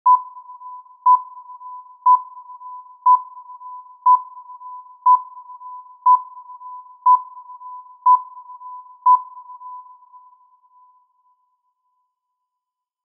Countdown Sound Effect (10 Seconds Beep Final Count Down) | FREE 4K DOWNLOAD - NO COPYRIGHT
10, 9, 8, 7, 6, 5, 4, 3, 2, 1, 0 ! Happy new Year. Epic Countdown Beep Sound Effect. 10 consecutive seconds reverb Beep Count Down.

Show, Opener or Youtube Video Countdown Music. Enjoy!